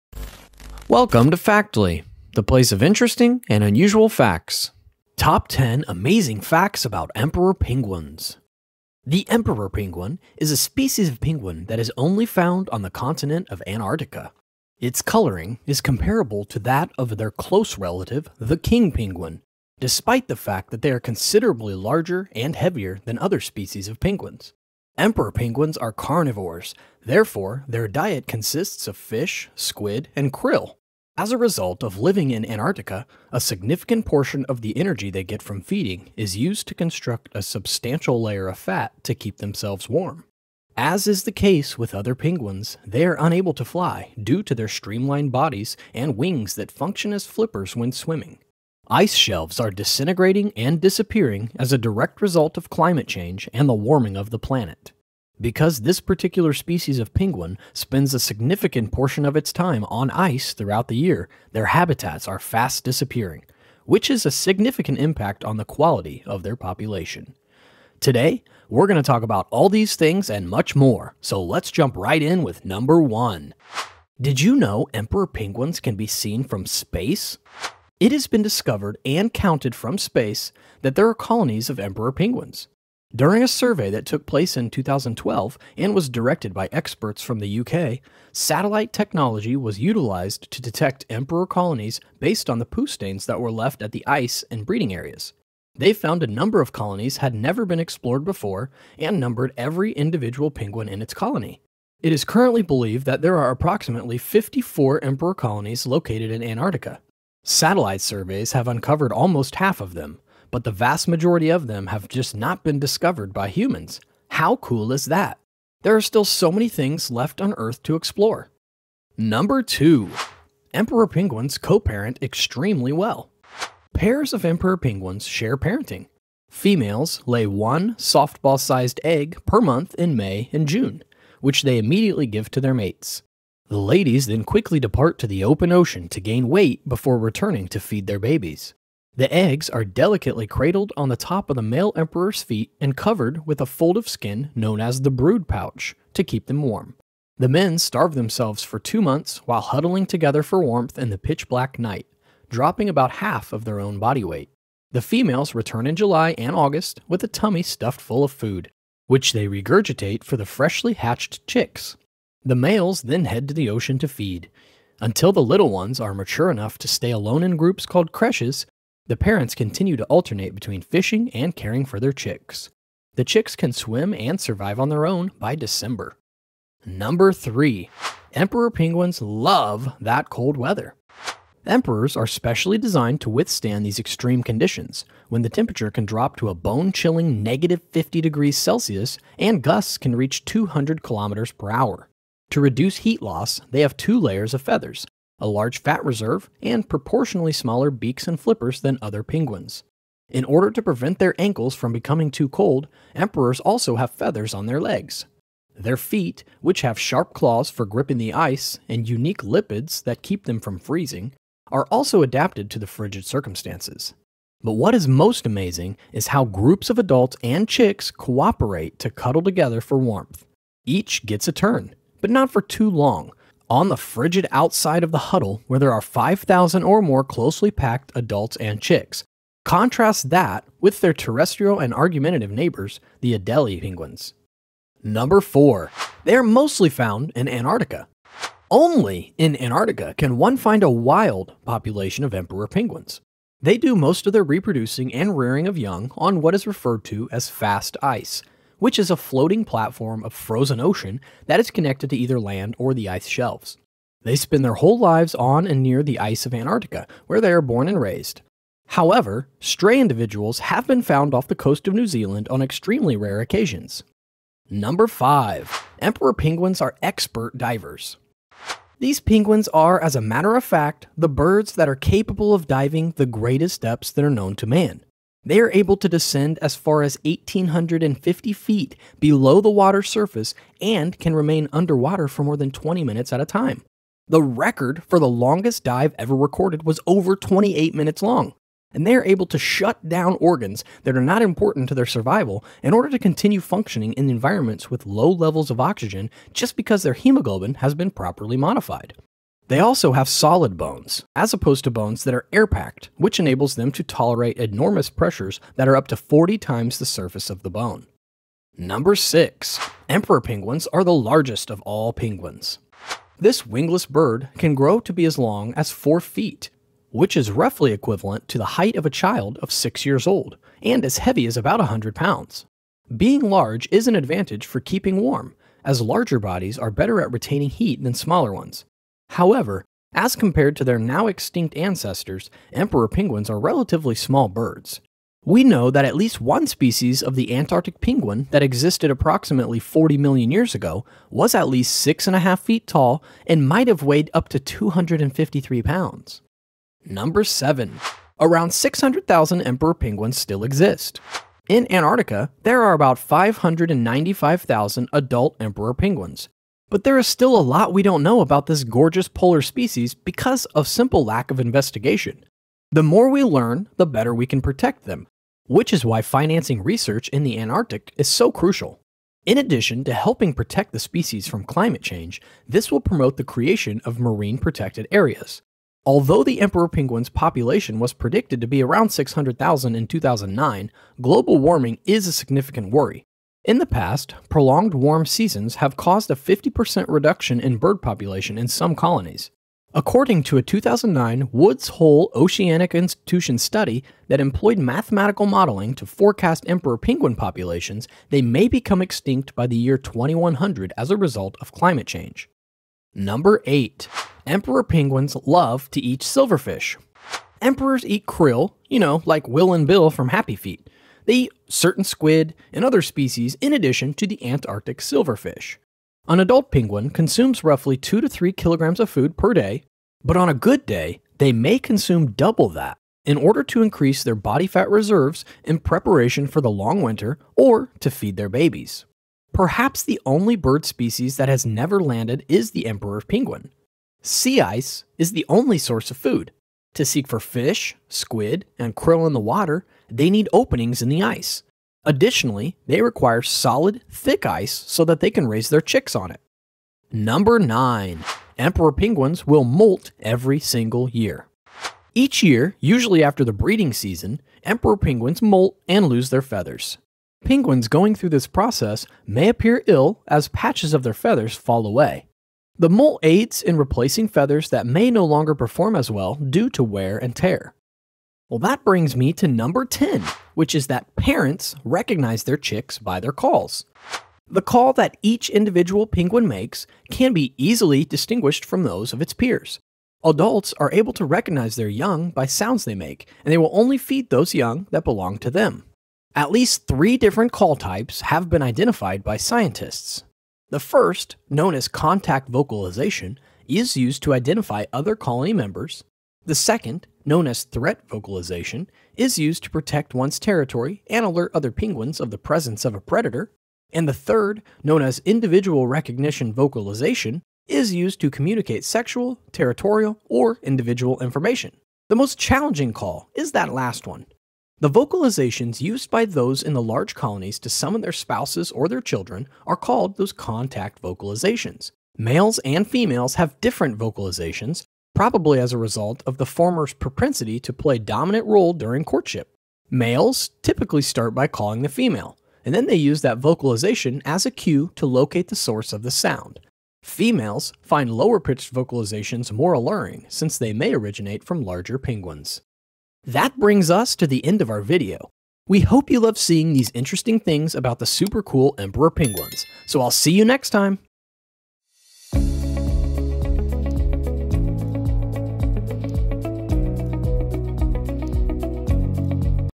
Top 10 Amazing Facts About Emperor Penguins
The emperor penguin is the tallest and heaviest of all living penguin species and is endemic to Antarctica. The male and female are similar in plumage and size, reaching 100 cm in length and weighing from 22 to 45 kg.